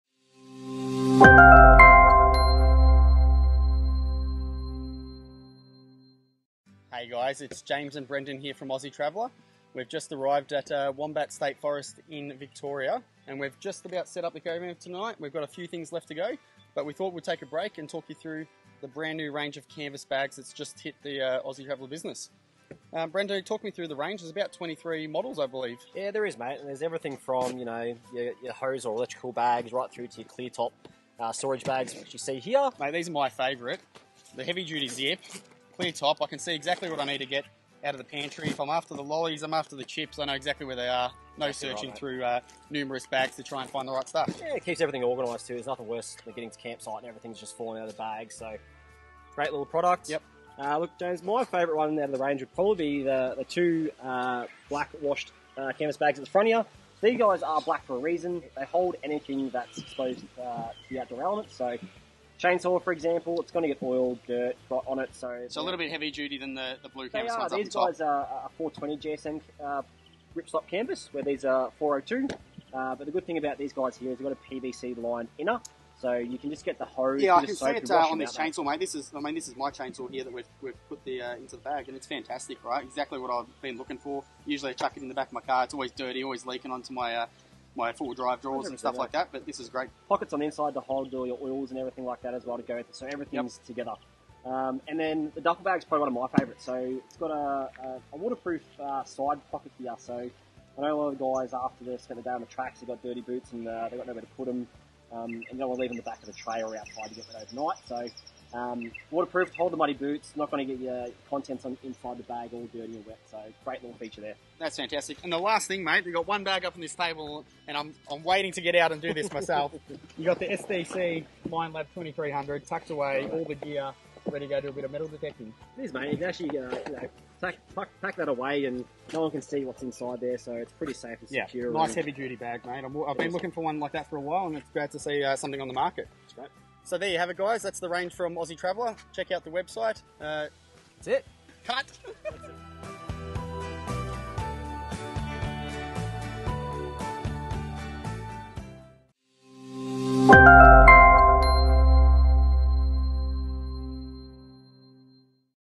A Look Into The Aussie Traveller Canvas Camping Bag Range
For more information on the Aussie Traveller Canvas Camping Bag Range,

Great storage can make or break a camping trip. Staying organised around your campsite has never been easier thanks to the new canvas camping bag range by Aussie Traveller. The new range features 23 types of canvas camping bag for your every camping need. Made from heavy duty ripstop polycotton canvas, every canvas camping bag is designed to last the distance – even in the harshest Aussie conditions. Choose from a huge range of sizes and styles, whether you want to store your hose or electrical gear, or keep your chainsaw protected when you’re out and about with a specially designed durable canvas bag. There’s even a cassette toilet bag, a spare wheel bag, and a pot bag for your camping pot! Every canvas camping bag features heavy duty zips so your valuables will stay protected. For extra durability, there are two black storage canvas bags featuring a 420 GSM ripstop canvas (compared to 402 GSM across the rest of the bag range) and a PVC lined inner making them watertight. Check out the full range of canvas camping bags at Caravan RV Camping.